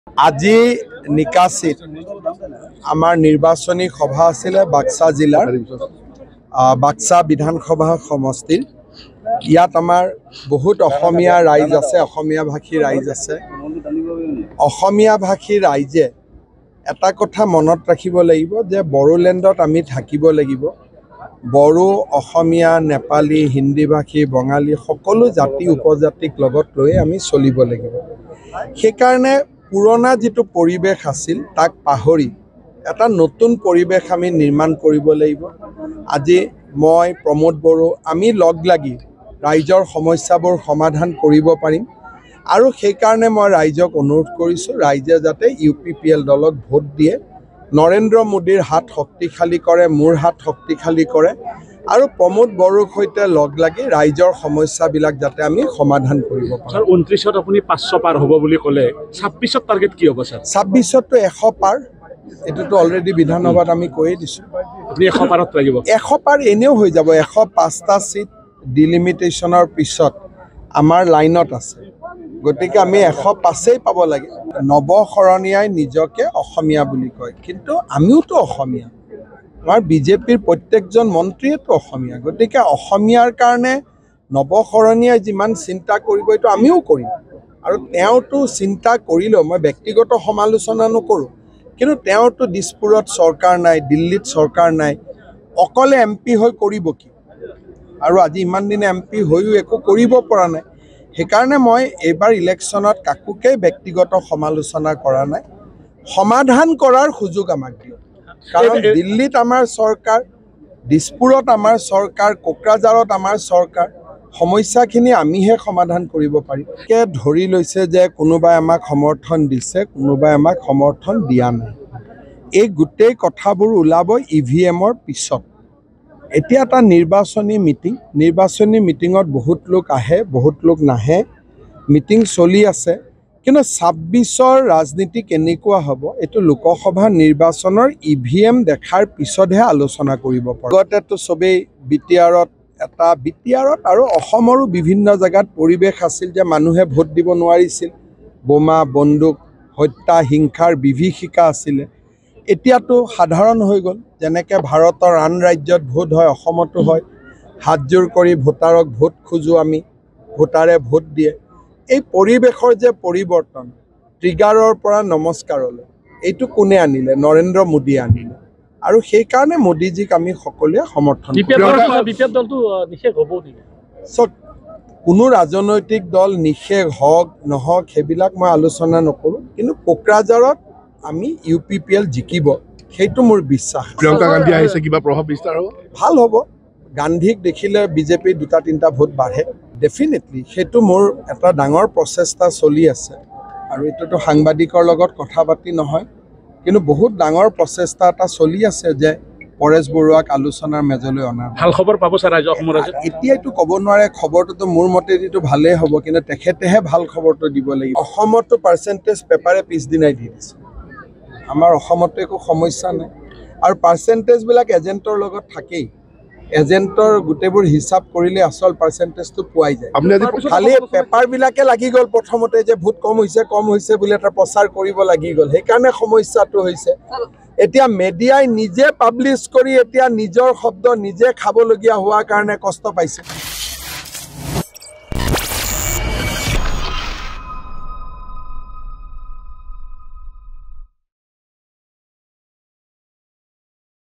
লোকসভা নিৰ্বাচন চলি থকাৰ মাজতে বিধানসভাৰ চিন্তা মুখ্যমন্ত্ৰী হিমন্ত বিশ্ব শৰ্মাৰ
'লোকসভাত বিজেপিৰ ৪০০ পাৰ, বিধানসভাত মিছন-১০৫।' ২০২৬ত অসমত ১০৫ আসন লক্ষ্য মুখ্যমন্ত্ৰীৰ। লোকসভা নিৰ্বাচন চলি থকাৰ মাজতে বিধানসভাৰ চিন্তা মুখ্যমন্ত্ৰী হিমন্ত বিশ্ব শৰ্মাৰ। মুছলপুৰত কোকৰাঝাৰৰ NDA প্ৰাৰ্থীৰ হৈ প্ৰচাৰ মুখ্যমন্ত্ৰী হিমন্ত বিশ্ব শৰ্মাৰ।